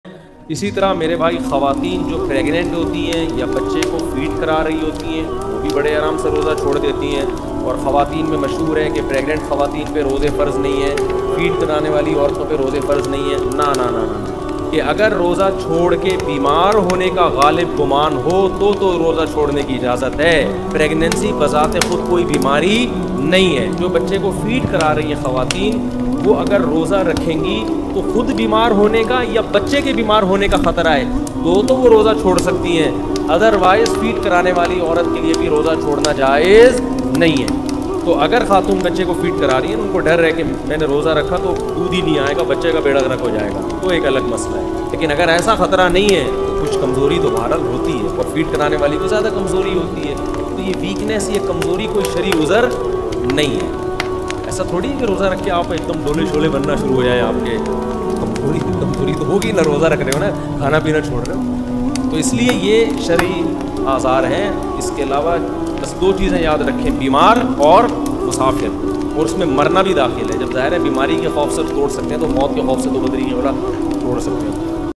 Kya Pregnant Aurten Roza chhod sakti hai? | Mufti Tariq Masood | Ramzan ke Masail
Pregnant aurton ka roza Mufti Tariq Masood